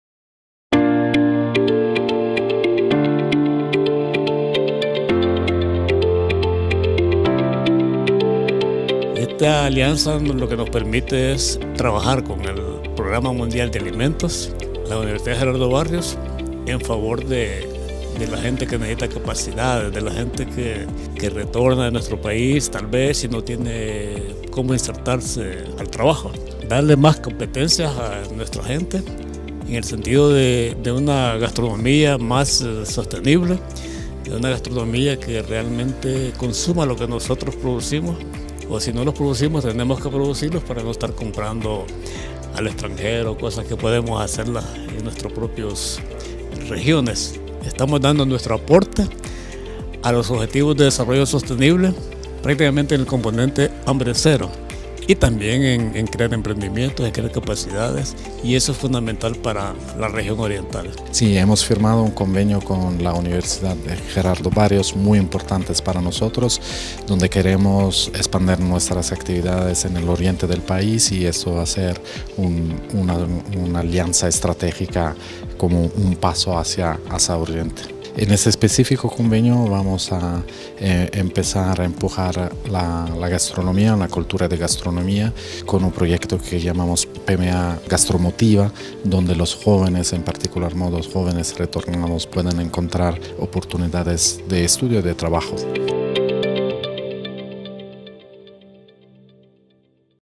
UGB firma convenio con el programa mundial de alimentos.
El pasado 10 de junio la Universidad Gerardo Barrios y El Programa Mundial de Alimentos de las Naciones Unidas firmaron un convenio de colaboración el cual busca conjuntar recursos y esfuerzos para crear e implementar conjuntamente los programas de formación culinaria y en arte urbano.